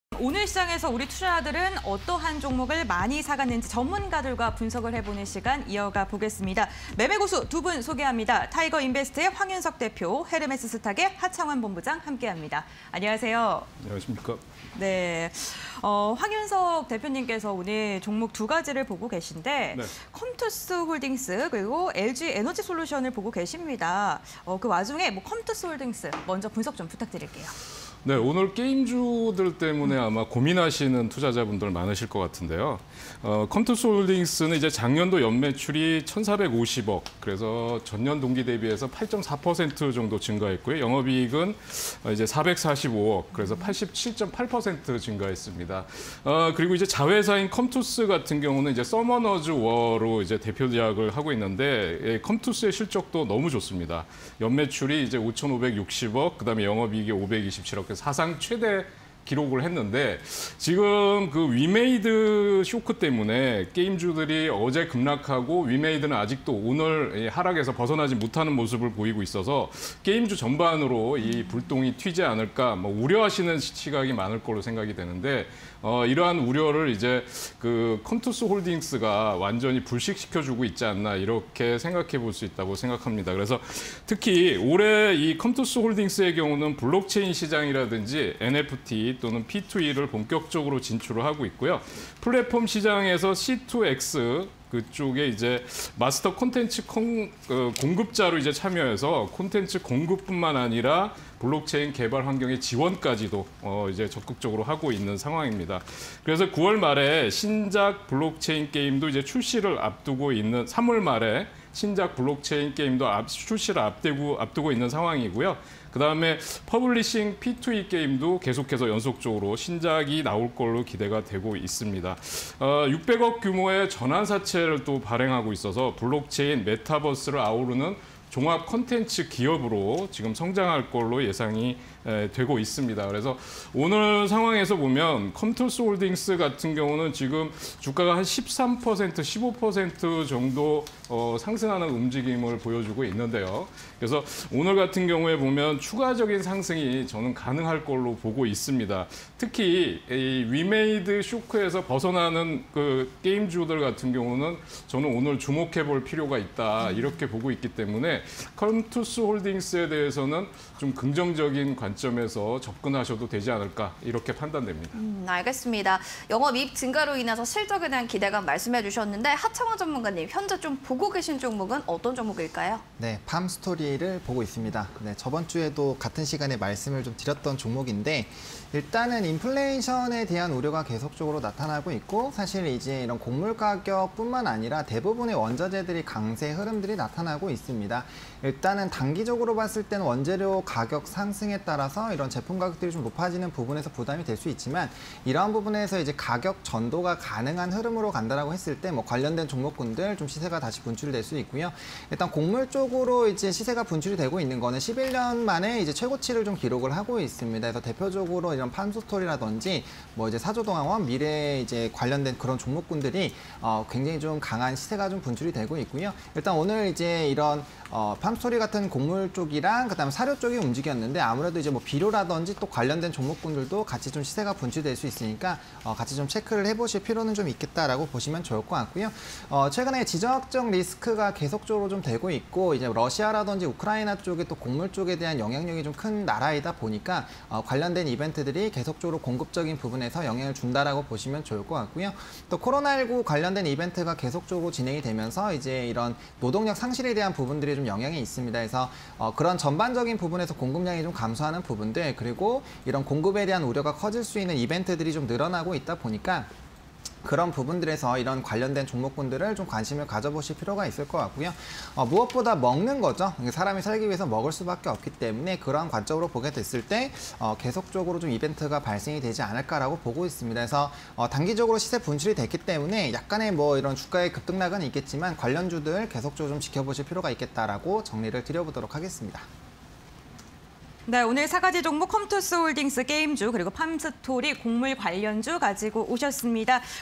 컴투스홀딩스, 팜스토리_사가지 (20220211)
컴투스홀딩스(063080)
- 지난해 영업이익 약 87.8% 증가
- 올해 블록체인ㆍNFTㆍP2E 본격 진출
- 암호화폐 '위믹스' 매도 금액, 매출에 반영

팜스토리(027710)
- 식량 가격, 2011년 이후 최고치
- 사료 가격 영향에 육류 가격 상승
- 기후변화ㆍ에너지 가격 상승 등 영향